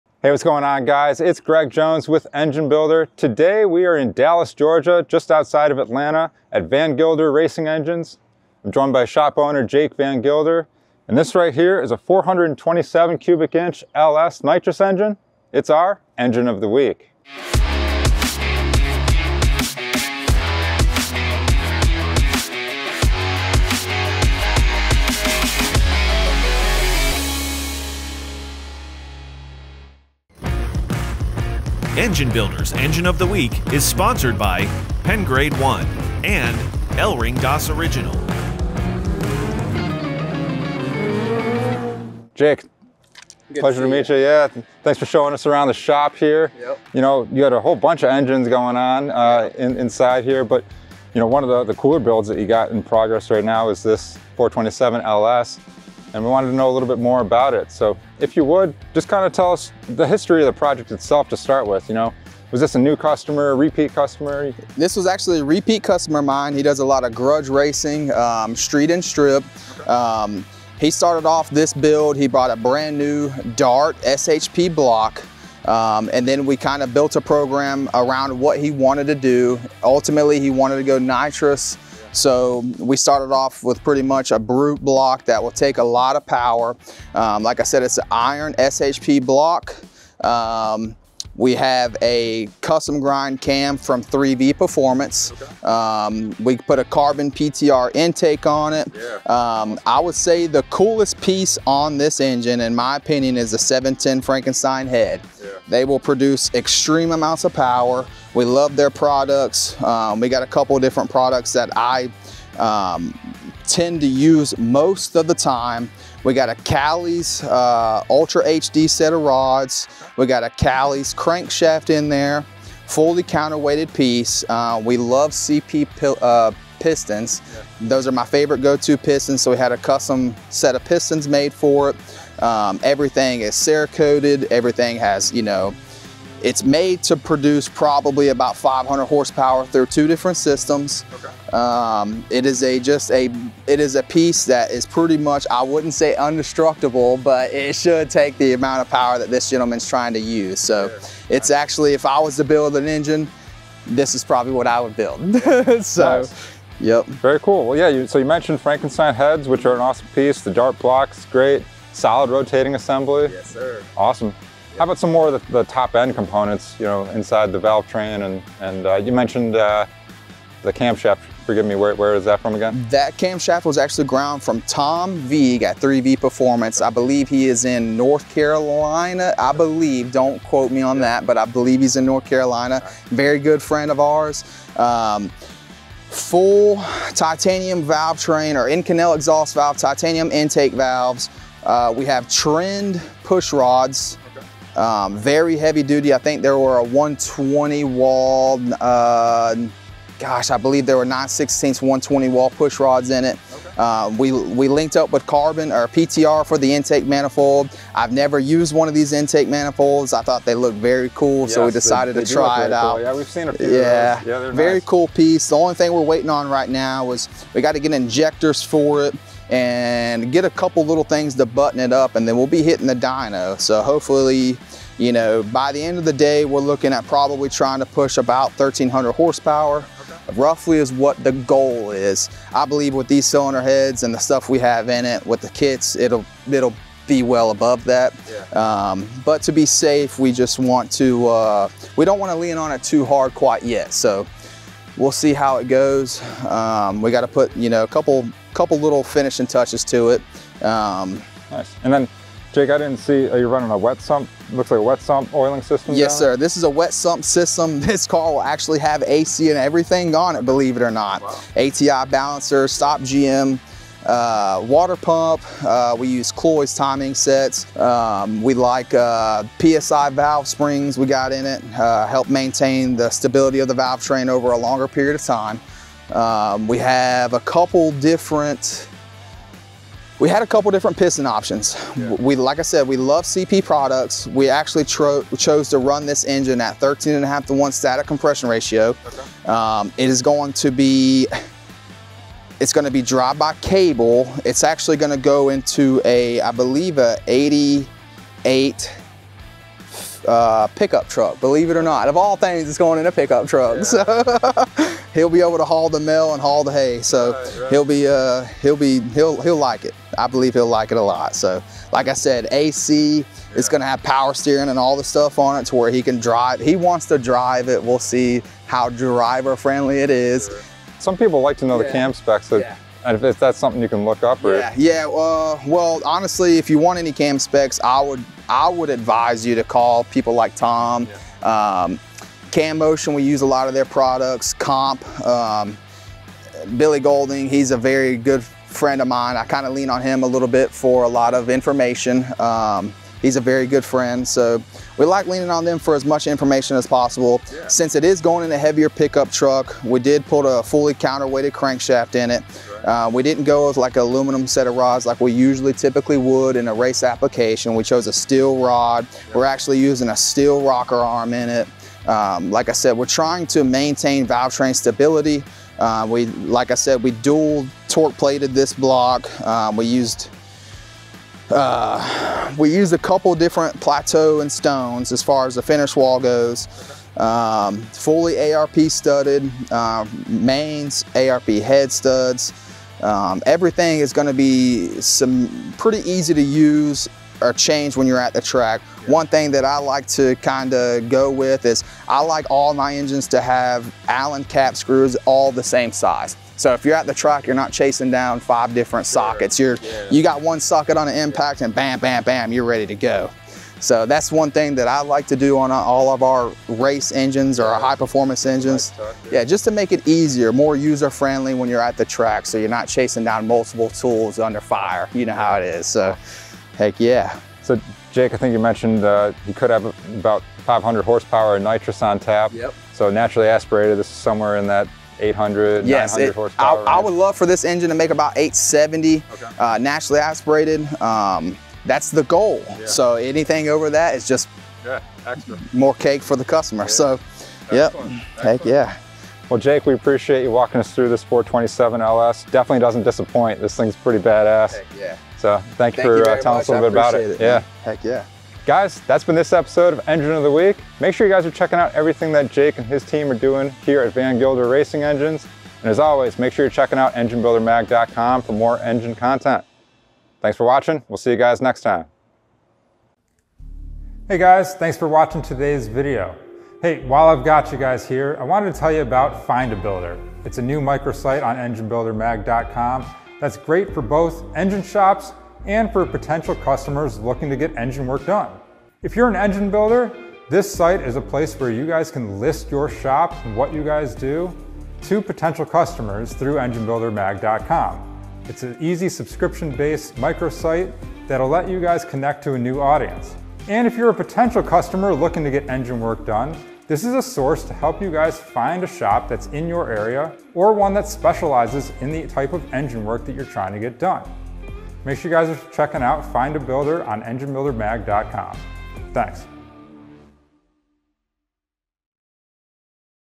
Van Gilder Racing Engines' 427 cid LS Nitrous Engine
Earlier this year, we spent a day out in Dallas, GA visiting Jake Van Gilder and his engine shop, Van Gilder Racing Engines. Jake does a little bit of everything when it comes to his engine builds, but while we were there, one of the cooler race builds was this nitrous-assisted 427 cid LS engine.